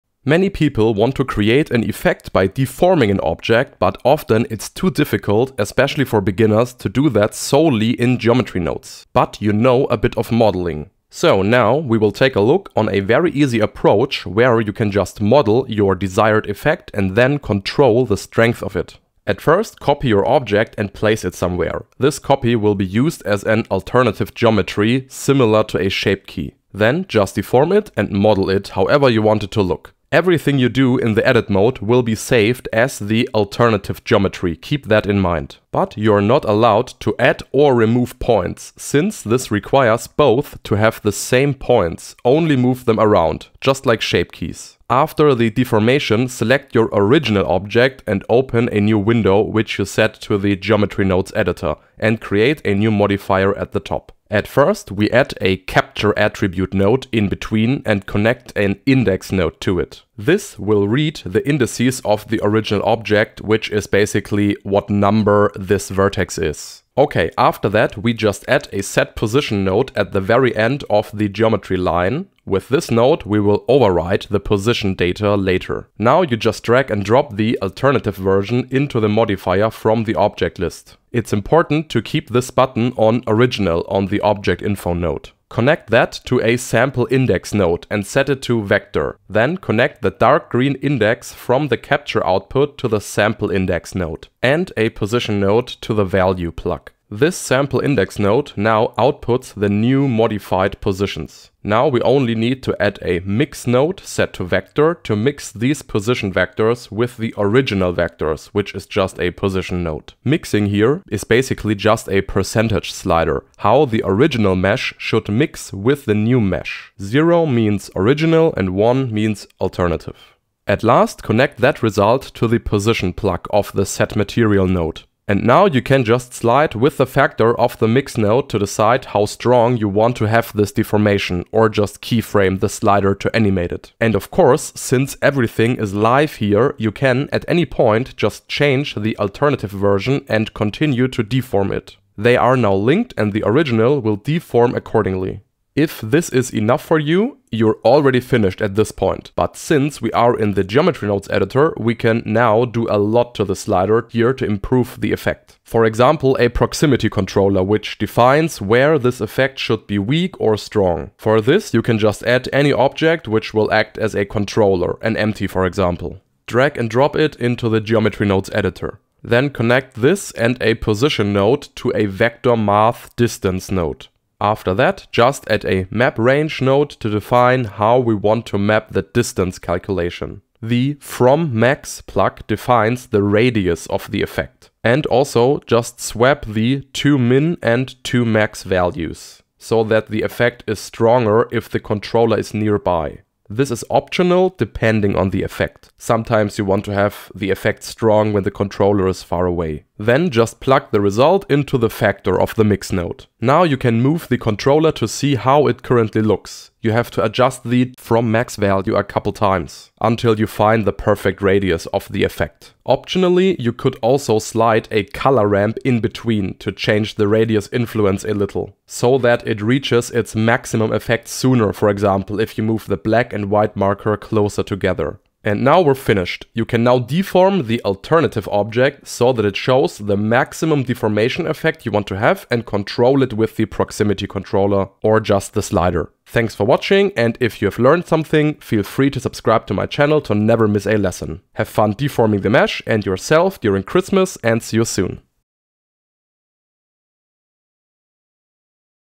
Finally ShapeKeys with Geometry Nodes!
Many Beginners are overwhelmed with deforming the mesh in the Geometry Nodes Editor, but this doesn't have to be like this.
In this video you will learn how to deform your object as usual in the edit mode and then just control that deformation with a controller to apply the strength of it and of course animate it.

or Fiverr: (a bit more expensive due to service fee)

Timestamps:
0:00 - Intro / Showcase
0:19 - Preparation (Duplicating and Deforming in Edit Mode)
0:44 - Geometry Nodes (Capture Original Indices)
1:15 - Geometry Nodes (Sample Alternative Geometry)
1:41 - Geometry Nodes (Mix Positions / Apply effect)
2:13 - Optional: Continue Deforming the Alternative Version
2:30 - Add Controller and drag it in the Editor
2:49 - Geometry Nodes (Distance to Controller)
3:56 - Outro